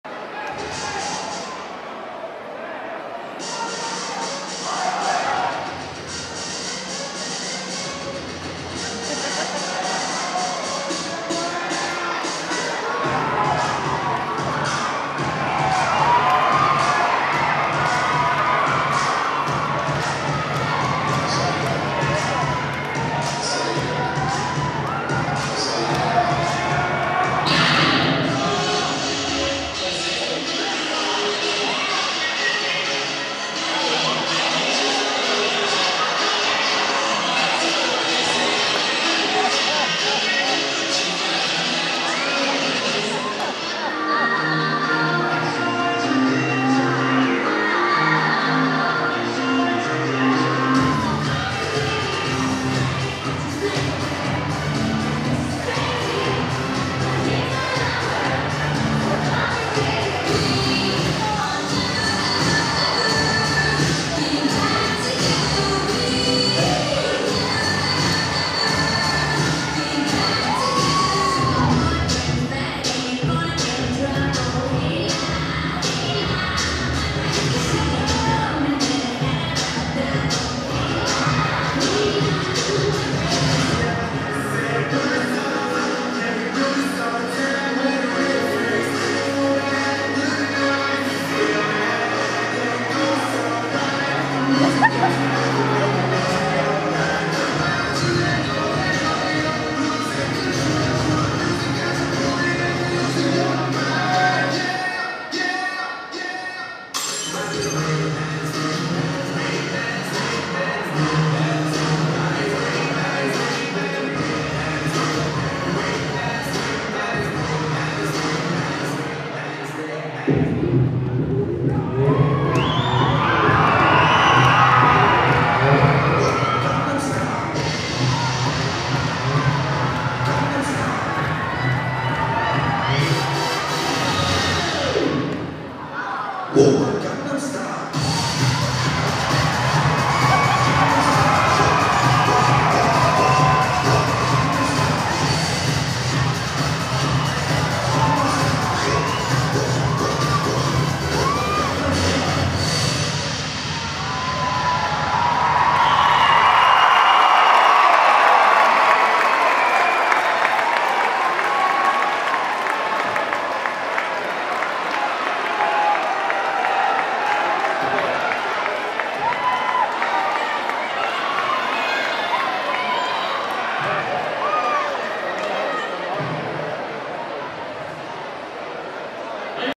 Pep Rally ML with Chris Paschal and Vic Davis
Traditional football players dance with Dance Team Members